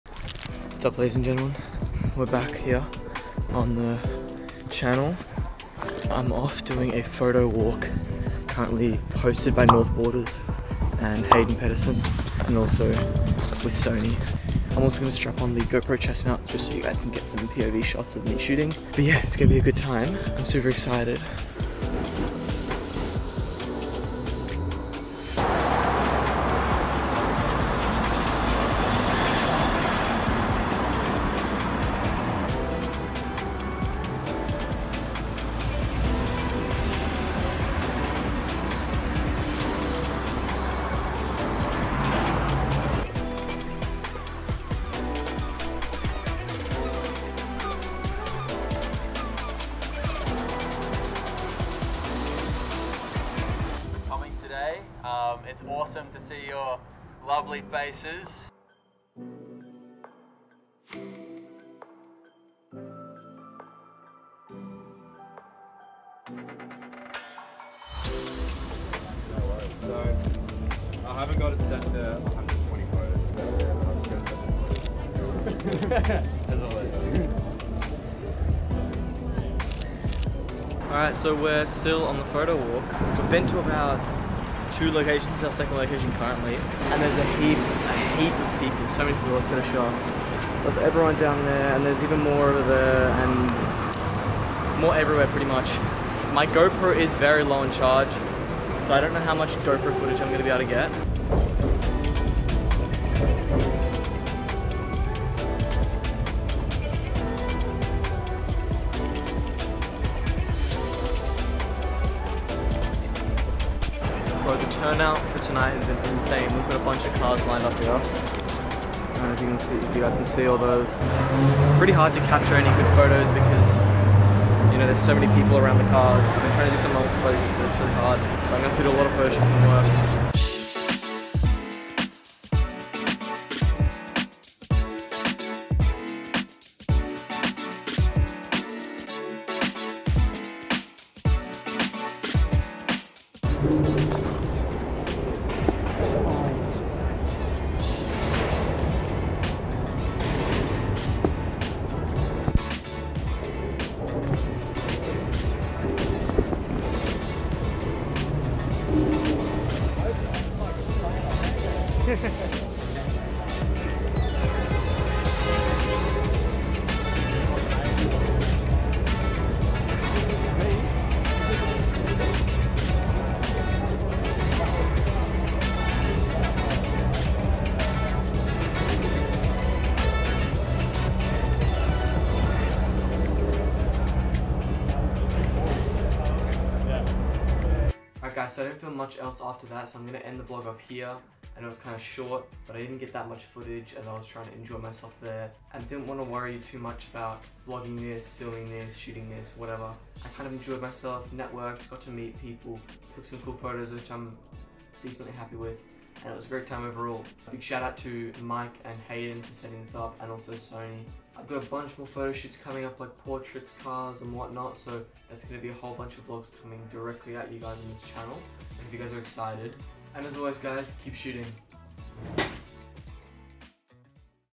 North Borders x Hayden Pedersen x Sony Photowalk 2024 Vlog
Hope you guys enjoyd this little vlog that I filmed whilst attending the North Borders x Hayden Pedersen x Sony photowalk 2024. This photowalk was such fun and was great meeting new people!

Gear...
Camera: Sony A7iii
Lens: Sony 85mm F1.8, Sony 28-70mm F/3.5-5.6
SD Card: SanDisk Extreme PLUS 256GB (200MB/s)
Tripod: gorilla tripod, Manfrotto advanced tripod